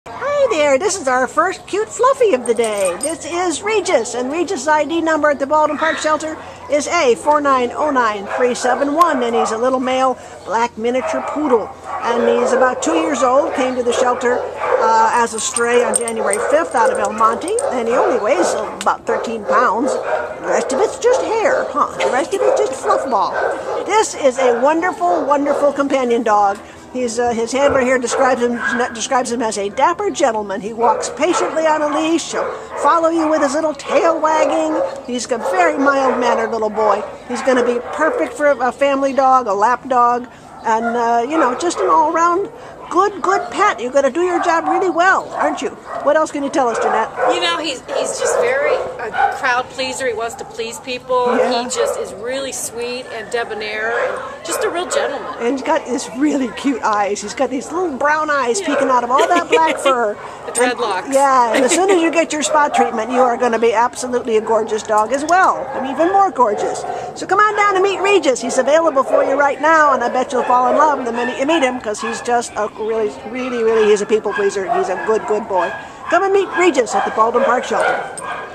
A4909371 Regis
A4909371 Regis is a debonair 2-year-old black male Miniature Poodle mix who was found as a stray in El  Monte on January 5th and brought to the Baldwin Park Animal Care Center. Weighing 13 lbs, Regis is a true people-pleaser: he is charming, smiley, and affectionate. Our volunteers describe him as “perfect” and “absolutely wonderful.” Regis likes other dogs, walks nicely on leash, and seems to be housebroken. His coat needs some TLC to have him looking his best, but with regular grooming, he will be gorgeous. Regis is a plug-and-play pet and will thrive as an indoor pet and companion in any loving household.